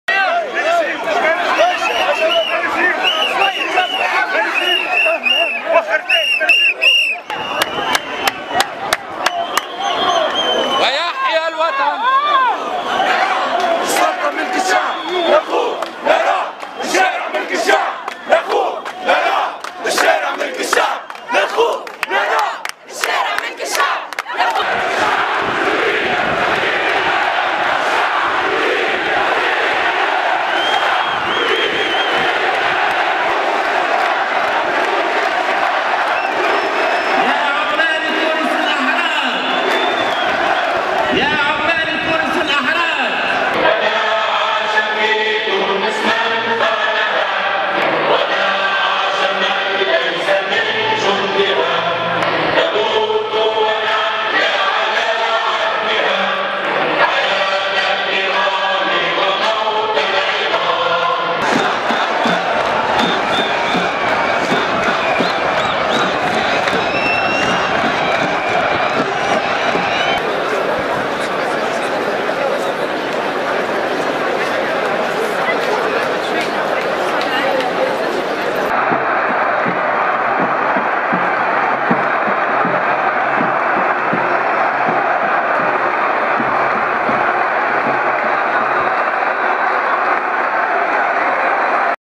Tunisian Revolutionary Symphony //// سمفونية ثورية تونسية
Thousands of Tunisians gathered in downtown Tunis to celebrate International Workers' Day. The first celebration began in front of the headquarters of the UGTT -- the eminent labor union in Tunisia -- in Mohamed Ali El Hammi Square.